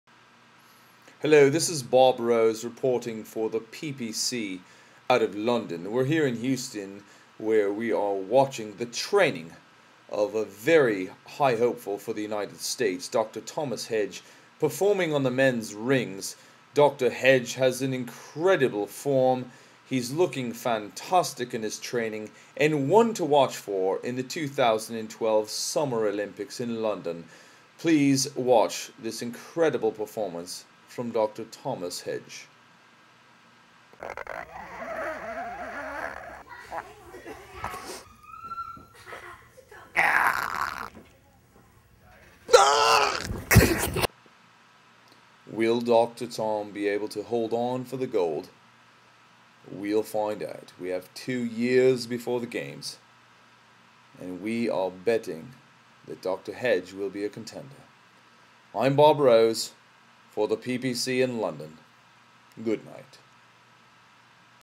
Dr. Tom Hedge - Gynmastics Houston - Olympic Hopeful for 2012 in London?
Dr. Tom Hedge Houston cosmetic dentist may be a contender in the 2012 Olympics in London. He shows his prowess on the rings in this never seen before training video.  Will he be able to hold on for the gold medal? Lets find out ... This behind the scenes video was captured at a gymnastics club in Houston, TX.  ONe thing for sure is he can craft gold medal smiles for anyone that wants a new smile.